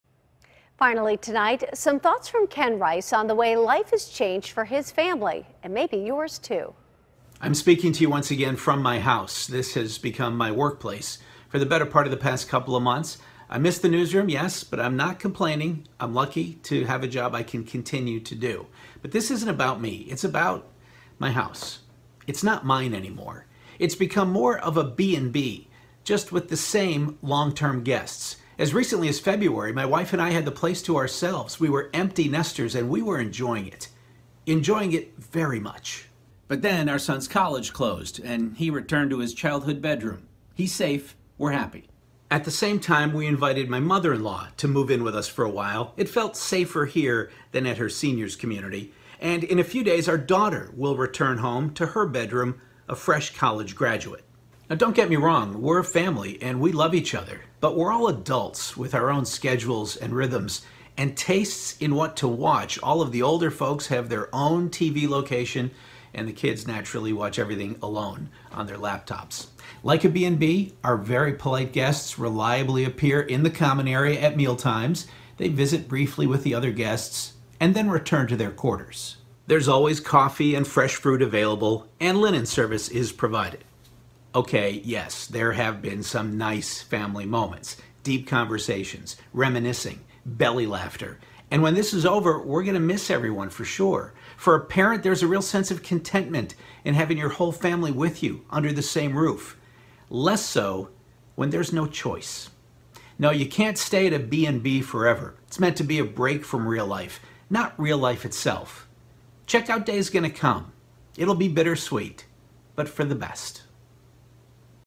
KDKA's Ken Rice Says His Home Has Changed During Coronavirus Pandemic
KDKA's Ken Rice says his house has become more of a bed and breakfast with the same long-term guests during the coronavirus pandemic.